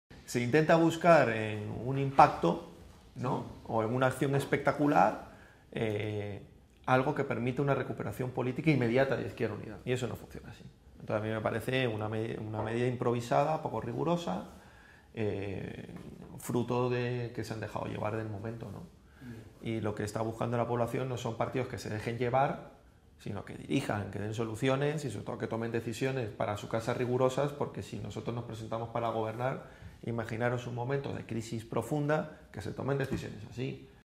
IU-Madrid rechaza la petición de dimisiones de Lara porque sólo persigue "una acción espectacular”
El coordinador de IU-Madrid, Eddy Sánchez, afirmó, al respecto de la decisión de la dirección federal de la coalición de pedir la dimisión de los anteriores coordinadores regionales por su responsabilidad política en la elección de José Antonio Moral Sántin como consejero de Caja Madrid, que “se intenta buscar un impacto o una acción espectacular que permita una recuperación política inmediata de IU”.

“Eso no puede ser así”, señaló Sánchez en una entrevista concedida a Servimedia.
“Me parece una medida improvisada y poco rigurosa, fruto de que se han dejado llevar por el momento”, señaló el líder regional de la coalición, quien consideró que “lo que está buscando la población no son partidos que se dejen llevar, sino que den soluciones y tomen decisiones rigurosas”.